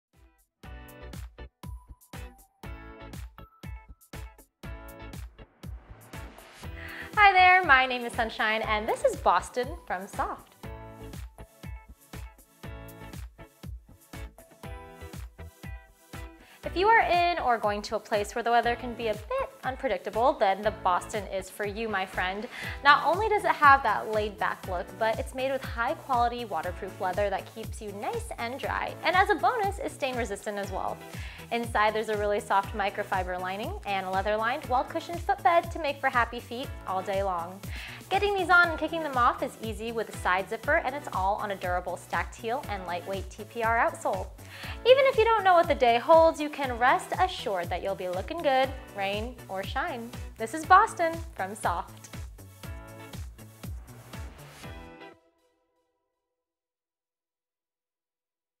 Söfft Shoes- The Bostyn
Wear the beautiful boots you love…without worrying about unexpected weather, when you slip on our gorgeous Bostyn moto boot!  Its fashionable slouch silhouette is crafted from waterproof leathers that resist stains and provide protection from moisture. Signature hardware adds the perfect finishing touch.

Offered in waterproof leather or waterproof oiled suede with leather accents
Seam-sealed construction
Microfiber lining
Leather-lined footbed, cushioned at ball, heel and arch for extra support
Stacked heel
Lightweight TPR non-slip outsole
Heel Height 1 ½ inches